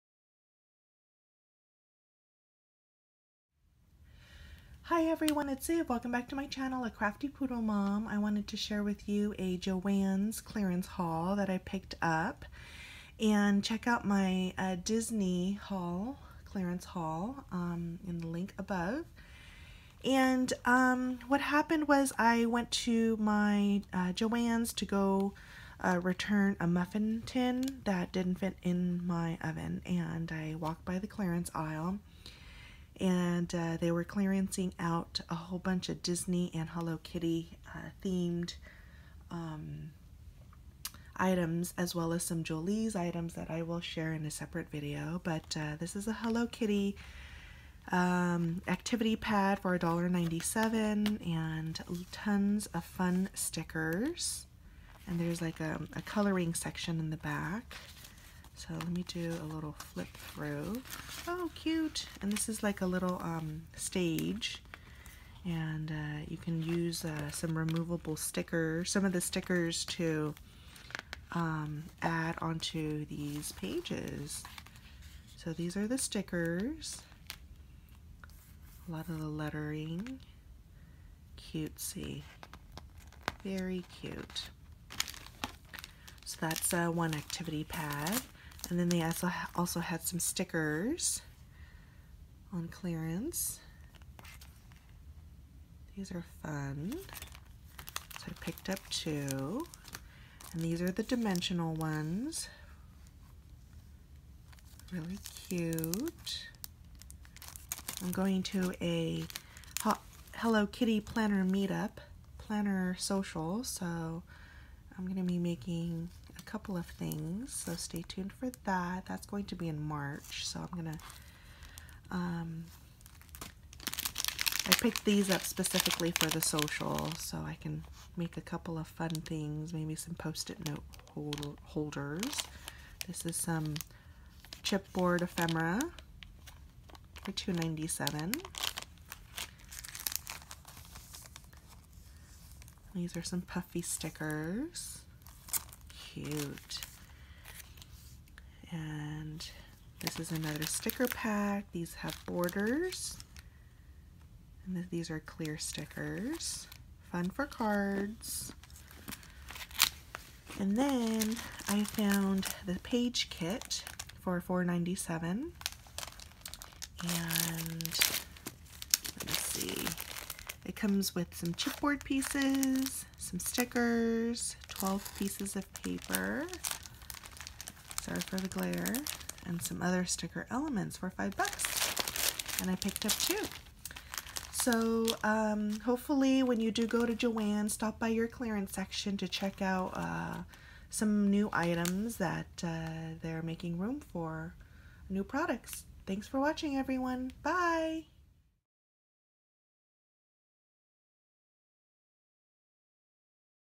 Hello Kitty Clearance Haul at JoAnn's Fabrics
I picked up these treasures on 2/4/17 at JoAnns.  Call them first if you are planning to stop by as not all JoAnns may be clearing out Hello Kitty items!

Happy hauling!

Hugs, Siv